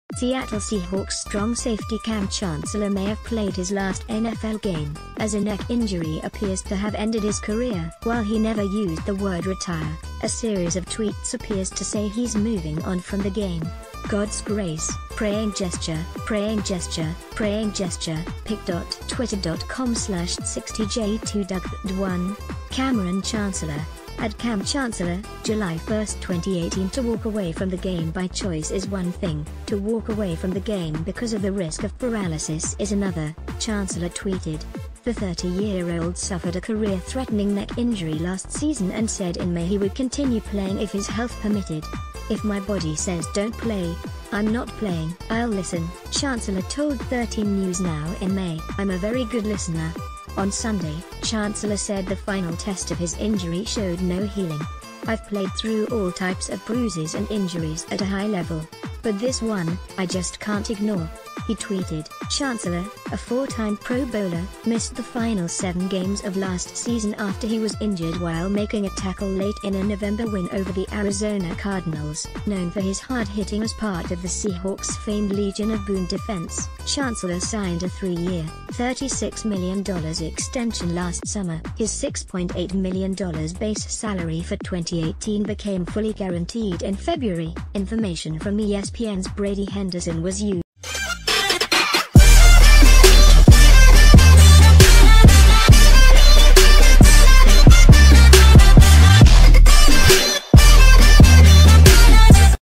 Kam Chancellor, Seattle Seahawks secure, announces retirement via Twitter
Seattle Seahawks strong safety Kam Chancellor may have played his last NFL game, as a neck injury ap...